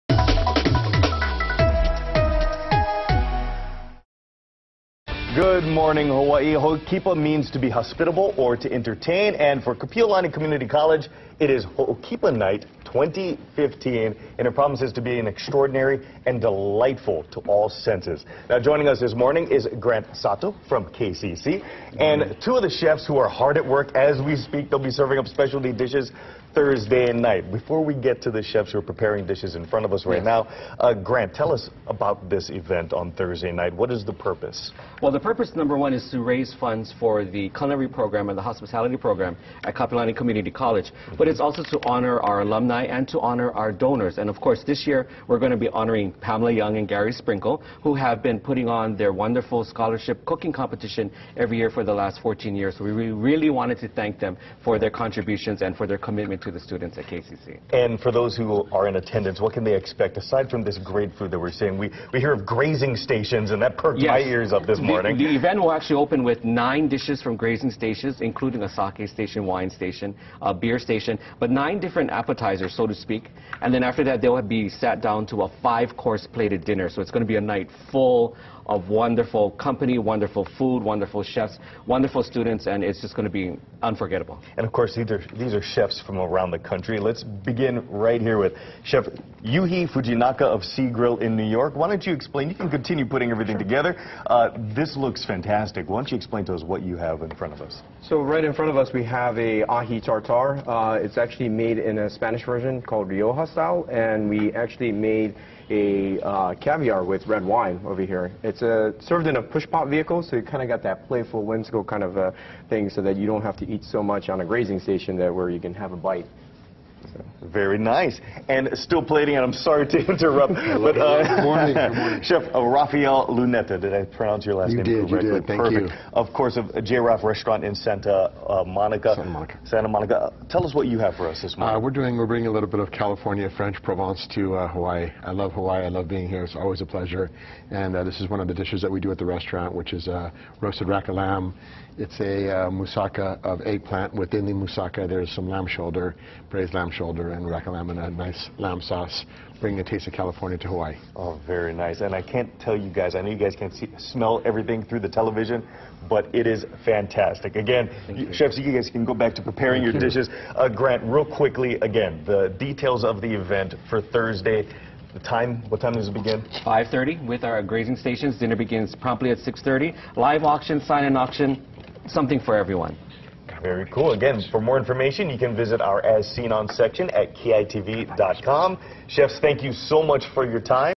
Kapiolani Community College features Hookipa Night
Get a taste of Hookipa Night 2015 this Thursday at the Hawaii Convention Center.